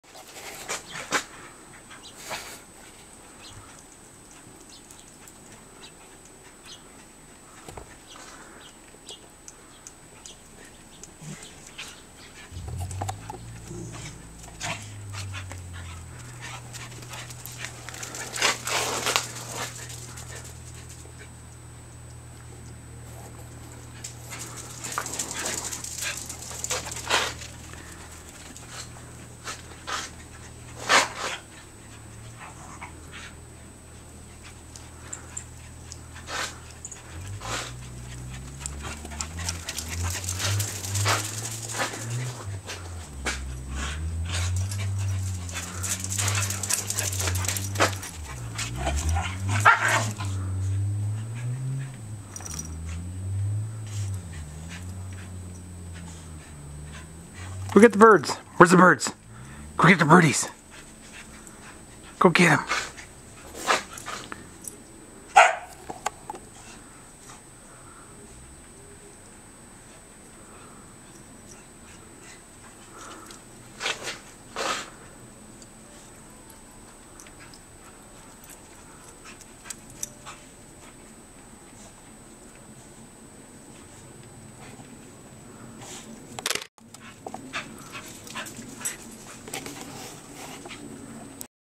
Corgi chase
Our dogs chasing in the snow